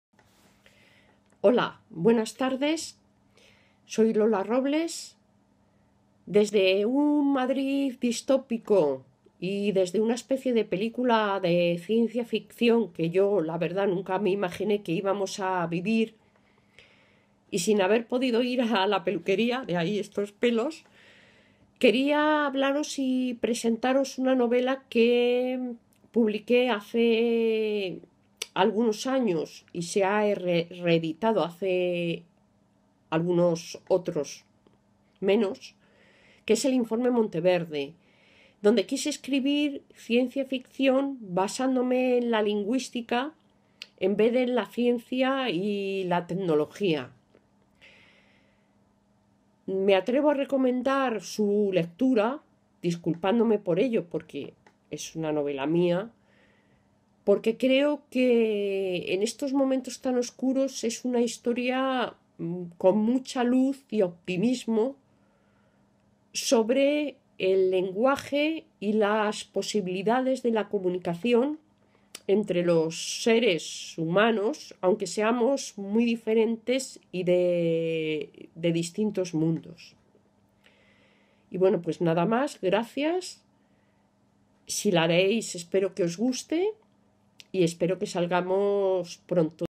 La UCO lee Ciencia | Lola Robles sobre 'El informe Monteverde'
La autora Lola Robles habla sobre su libro 'El informe Monteverde' para la actividad 'La UCO lee ciencia' organizada por la Unidad de Cultura Científica y de la  Innovación de la UCO